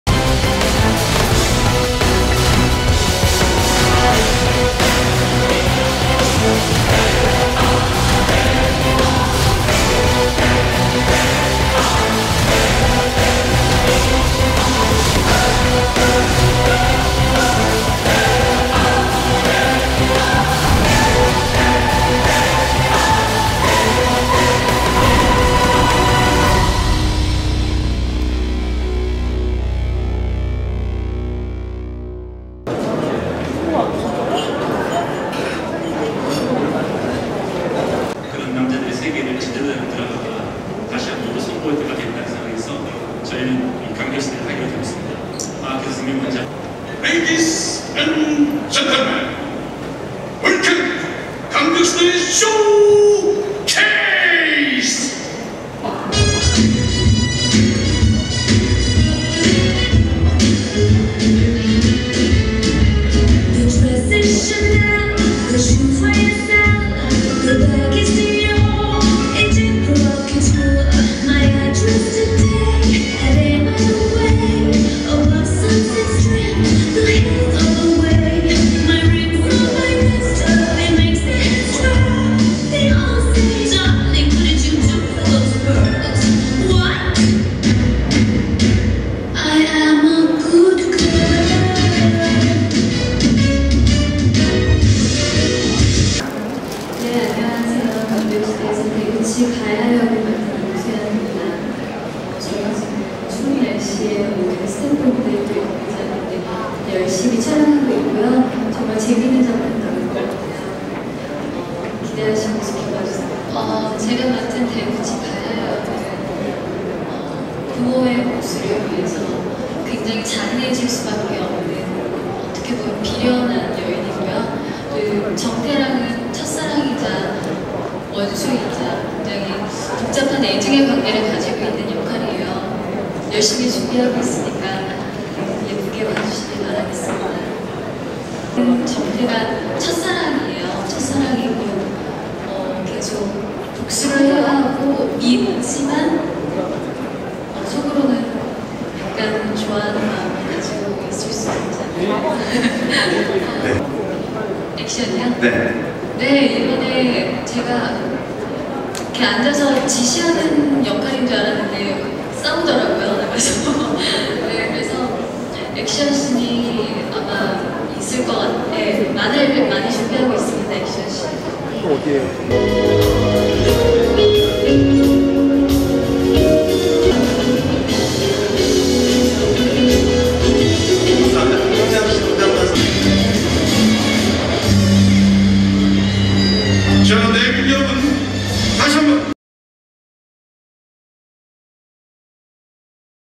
[눈TV] `감격시대` 임수향 "앉아서 지시하는 역할인 줄 알았는데..."
(서울=News1) 조한결 인턴기자=KBS2 새 수목드라마 '감격시대'에 출연하는 배우 임수향이 "앉아서 지시하는 역할인 줄 알았는데 (직접) 싸우더라"며 드라마에서 화려한 액션을 선보일 것이라고 말했다.

임수향은 지난 2일 서울 강남구 라움 마제스틱볼룸에서 열린 드라마 '감격시대' 쇼케이스에서 '아이리스2'에서처럼 화려한 액션을 보여주는지 묻는 질문에 "이번엔 제가 앉아서 지시하는 역할인 줄 알았는데 (직접) 나가서 싸우더라"며 "액션신을 많이 준비하고 있다"고 밝혔다.

그는 '아이리스2'에서 아이리스 소속 킬러 김연화를 연기하며 화려한 액션신을 선보여 호평받은 바 있다.

이날 쇼케이스에 기모노 차림으로 등장한 임수향은 '감격시대'에서 조선인 아버지와 일본인 어머니 사이에서 태어난 데쿠치 가야 역을 맡았다. 극 중 데쿠치 가야는 정태(김현중 분)의 아버지에 의해 자신의 아버지를 잃으면서 정태에게 복수의 감정과 애정을 함께 가진다.

한편 이날 오프닝 무대에서 데쿠치 가야의 어린 시절을 연기하는 배우 주다영이 1930년대 풍의 춤을 선보이며 임수향과 놀라운 싱크로율을 보여줘 눈길을 끌었다.

'감격시대'는 150억원이 넘는 제작비가 투입되는 거대한 스케일의 드라마로 1930년대 한, 중, 일 주먹들이 펼쳐내는 사랑과 의리, 우정 등을 그린다.

임수향 외 김현중, 진세연, 조동혁, 김갑수 등이 출연하며 2014년 1월 방영될 예정이다.